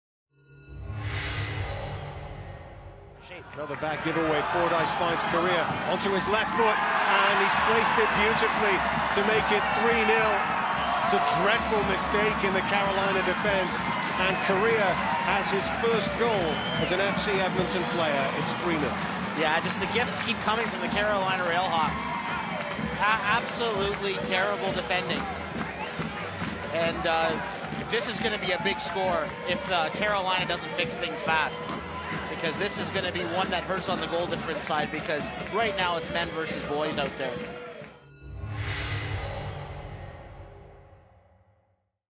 Eddies' Dustin Corea scores his first FC Edmonton goal!
Dustin Corea of FC Edmonton scores his first goal as an Eddie as he gives his team a 3-0 lead vs. the Carolina RailHawks at Clarke Stadium in Edmonton AB on August 16, 2015.